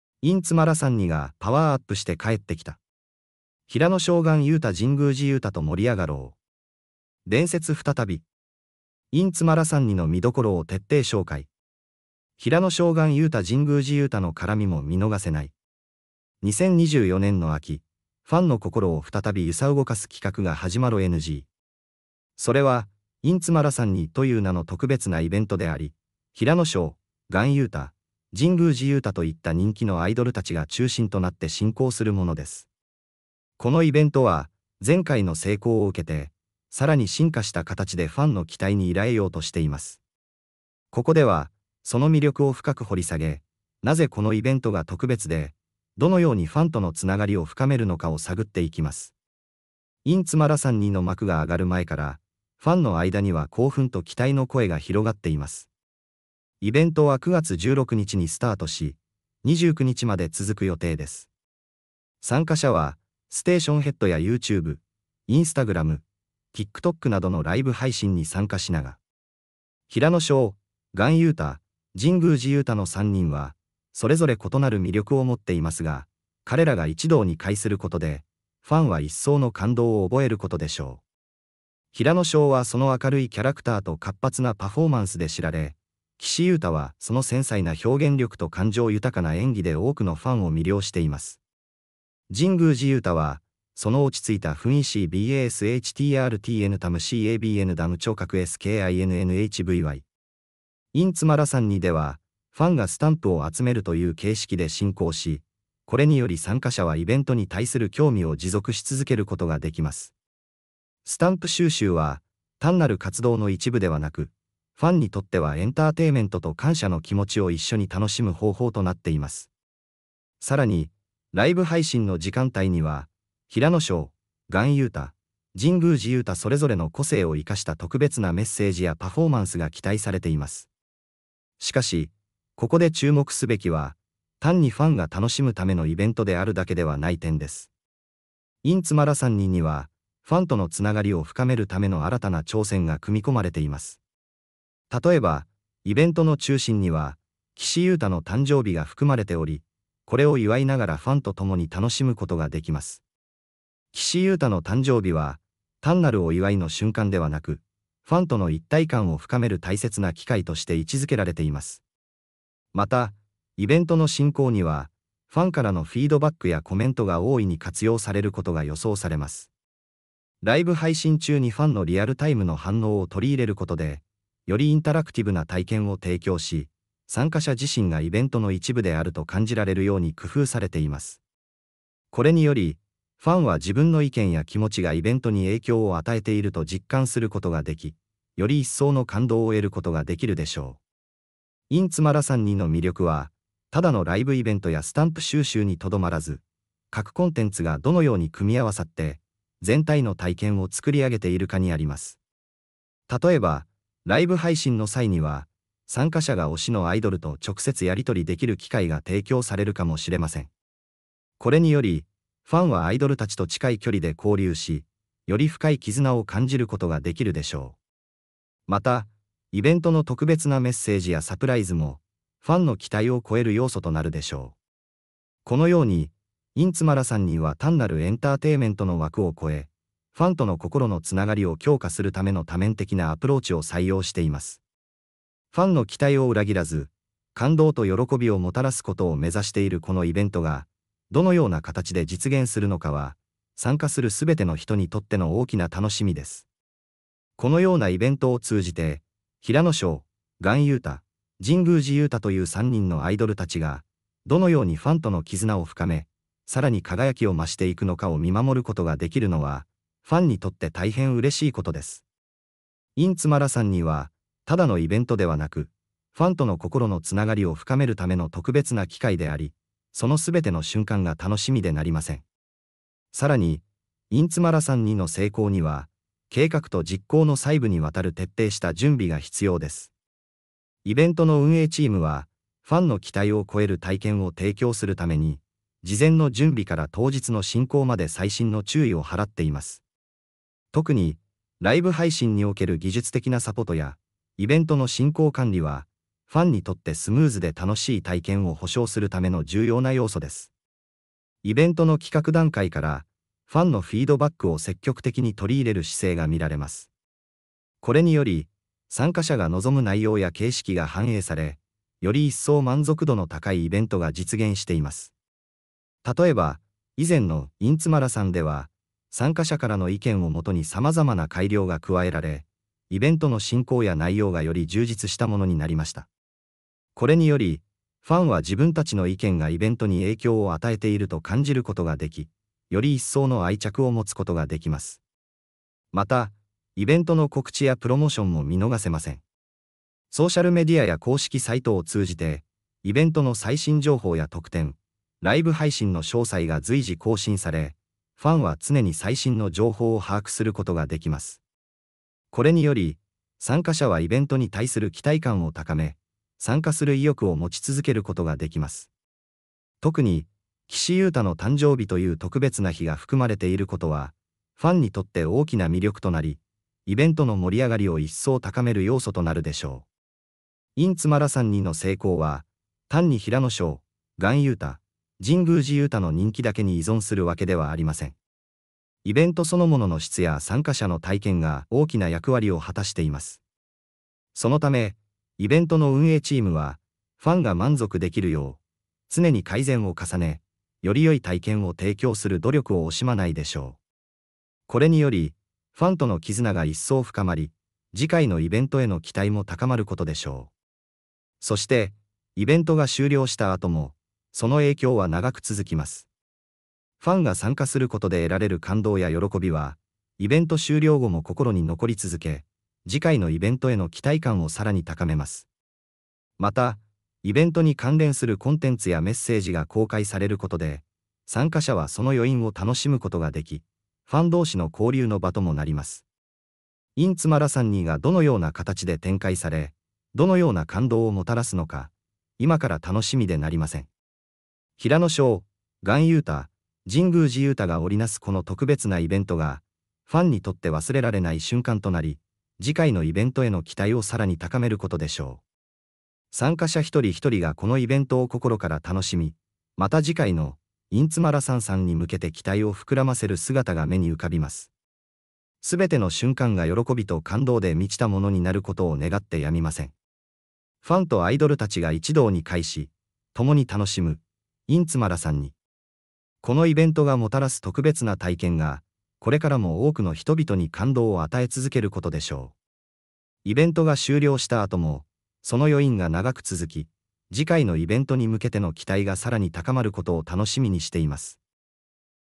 【平野紫耀】INZMARATHONⅡがパワーアップして帰ってきた！平野紫耀・岸優太・神宮寺勇太と盛り上がろう！伝説再び！INZMARATHONⅡの見どころを徹底紹介！平野紫耀・岸優太・神宮寺勇太の絡
【平野紫耀】INZMARATHONⅡがパワーアップして帰ってきた！平野紫耀・岸優太・神宮寺勇太と盛り上がろう！伝説再び！INZMARATHONⅡの見どころを徹底紹介！平野紫耀・岸優太・神宮寺勇太の絡みも見逃せない！